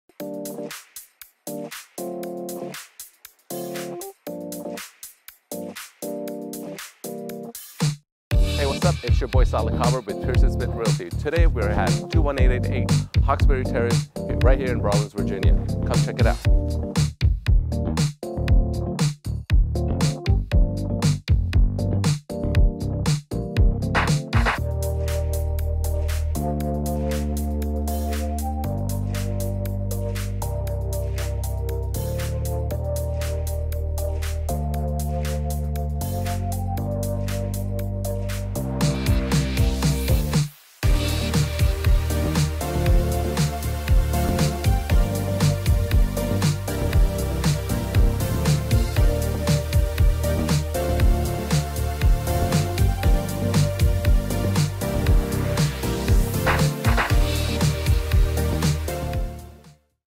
21888 Hawksbury Ter, Broadlands, VA 20148
Beautiful 4 bedroom 3 ½ bath town home in highly sought-after Broadlands South. This home has been meticulously maintained and ready for you to call it home. You are greeted with hardwood floors in the foyer, taking a few steps you are in an open living and dining room combo which has built-in surround sound speakers, crown molding and much more. Kitchen has an island and space for a table with a gas cook top and double wall oven. The eat-in kitchen walks out to the deck and adjoins the additional cozy family room. Follow the carpeted floors upstairs where you will find 3 spacious bedrooms. Primary BR has vaulted ceilings and walk in closet, large soaking tub. Laundry is also on the top floor for convenience. Basement is carpeted and has an outside access along with 4th bedroom and a full bath. Walk out on the lower level to a custom patio and a fully fenced in backyard—perfect for outdoor entertaining. HVAC replaced 2020. HOA INCLUDES HIGH SPEED INTERNET. Broadlands is one of best locations in Ashburn—just minutes from VA-267, RT 7 and RT 28 and future home to the silver line. Plenty of shopping centers nearby for grocery and restaurants. Close to One Loudoun and Brambleton Town Center. Professional pics coming soon.

Listed By Saliq Khawar of Pearson Smith Realty